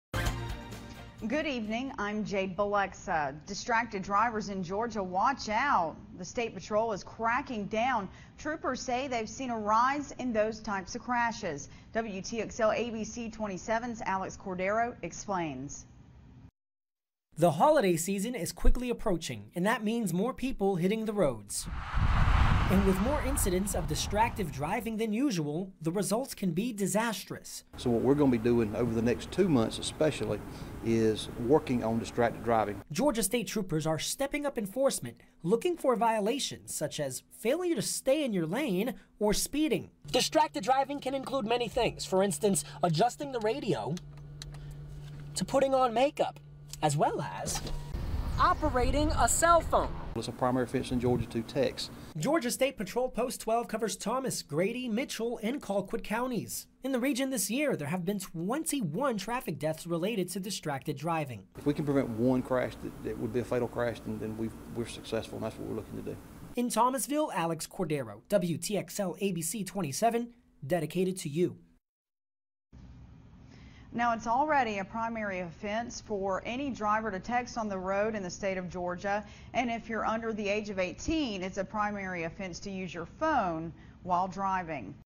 Georgia State Patrol Looking for Distracted Drivers
The Georgia State Patrol is cracking down on distracted driving